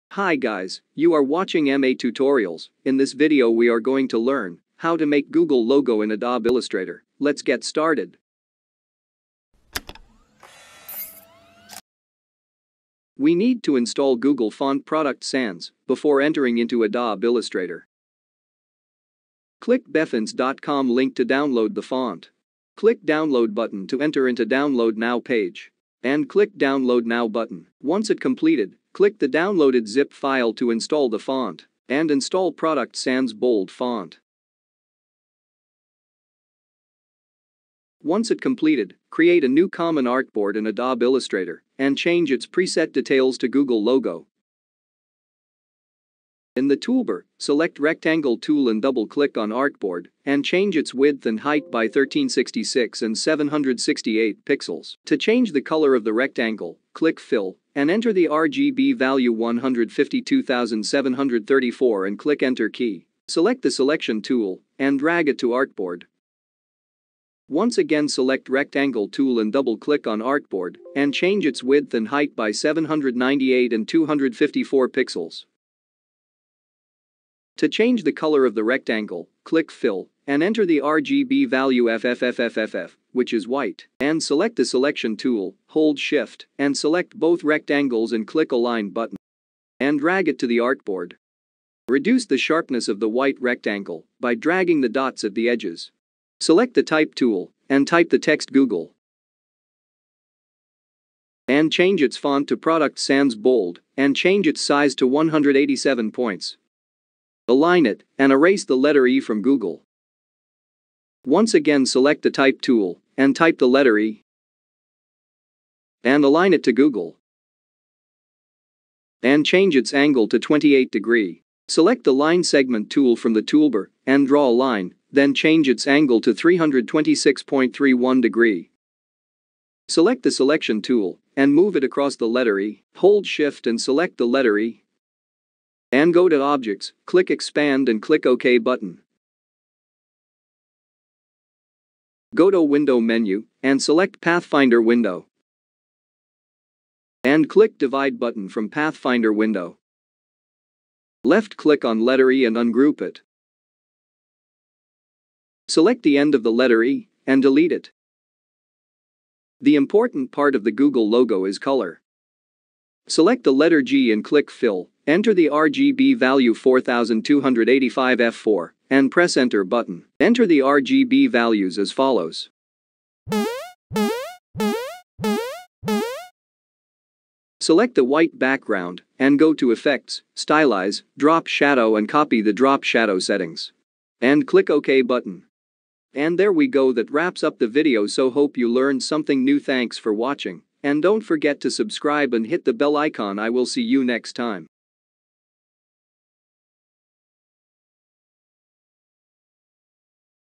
how to create google logo in illustrator
Hi guys.
Welcome to M A Tutorials.
This tutorial will demonstrate how to design a Google logo. The techniques featured in this video use a variety of tools and panels.
Even beginners can create this design without interruption.